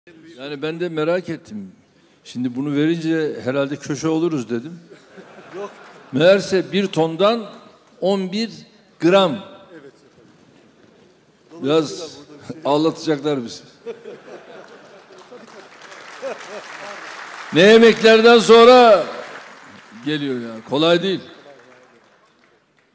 Başkan Erdoğan altın madeni tesisinin açılışında espriyi patlattı: Köşe oluruz dedim ama...
Bilecik'te Söğüt Altın Madeni'nin açılışını yapan Cumhurbaşkanı Erdoğan'ın, kendisine hediye edilen külçelerle ilgili yaptığı esprisi salonu kahkahaya boğdu. Erdoğan, ""Bunu alınca köşe oluruz dedim. Meğerse 1 tondan 11 gram çıkıyormuş. Biraz ağlatacaklar bizi" sözleriyle güldürdü.

Bilecik'te keşfedilen 109 ton altının ekonomiye kazandırılması için geri sayım başladı. Cumhurbaşkanı Erdoğan da bugün Bilecik'teydi ve Söğüt Altın Madeni'nin açılışını yaptı. Açılışta bir konuşma yapan Erdoğan, hem altındaki hedefi açıkladı, hem de bugüne kadar yapılan yatırımlardan bahsetti.

AÇILIŞTA ESPRİYİ PATLATTI

Açılışta renkli anlar da yaşandı. Maden yatağından çıkarılan, "Akbaştepe" ve "Korudanlık" adı verilen külçelerin hediye edildiği Cumhurbaşkanı Erdoğan, espriyi patlattı.

"BUNU ALINCA KÖŞE OLURUZ DEDİM"

Külçelerden birini eline alan Erdoğan, "Bunu alınca köşe oluruz dedim. Meğerse 1 tondan 11 gram çıkıyormuş. Biraz ağlatacaklar bizi" ifadelerini kullandı. Erdoğan'ın sözleri açılışa katılanları güldürdü.

"YILLIK ÜRETİM MİKTARINI 6,5 TONA YÜKSELTECEĞİZ"

Açılışını yaptığı altın madeni ile ilgili de bilgi veren Erdoğan, "Söğüt Altın Madeni, hayırlı olsun. Maden sahasında yapılan çalışmalar neticesinde 109 ton altın kaynağıyla 60 ton altın rezervi tespit edildi. Bir başka ifadeyle 3,5 milyon ons kaynağı altının bir an önce Türk ekonomisine kazandırılması için çalışmaları hızlandırdık. Maden sahasındaki faaliyetler 3 faz olarak 500 milyon dolara ulaşacak şekilde planlandı. 70 milyon dolar yatırım gerçekleştirilerek projenin ilk kısmı tamamlandı. Tesis ilk altın dökümünü yapacak şekilde hazır hale getirildi. Kalan iki fazın yatırımlarını 3 yıl içinde neticelendirmeyi planlıyoruz. Yıllık üretim miktarını 6,5 tona yükselteceğiz. Burası tam kapasite ile faaliyete geçtiğinde ülkemizde en çok altın üretimi yapılan ilk 3 madenden birisi olacaktır. Yaklaşık 1000 kardeşimiz istihdam ediliyor. İlerleyen dönemlerde bu sayının 1300'ü bulmasını bekliyoruz" dedi.